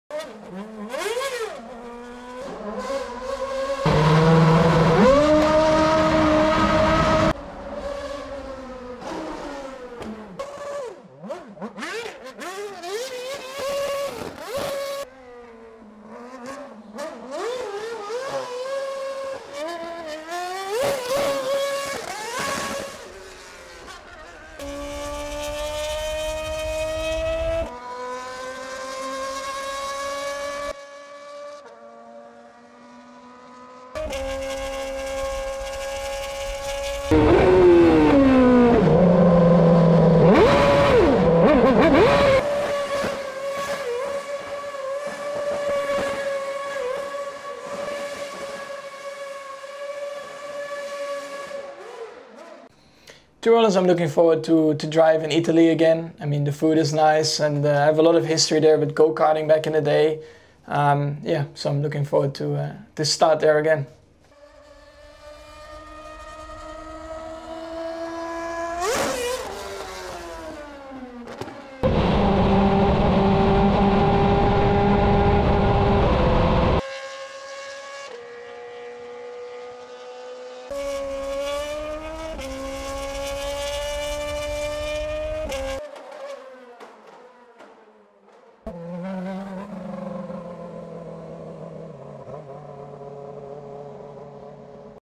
Ｆ１イタリアＧＰを前にフェルスタッペンがパレルモの街をドライブ
今週末開催される自動車レースＦ１のイタリア・グランプリ（ＧＰ）を前に、個人総合で首位のマックス・フェルスタッペン（レッドブル・ホンダ）が、イタリア・シチリア島パレルモの街をＦ１カーでドライブした。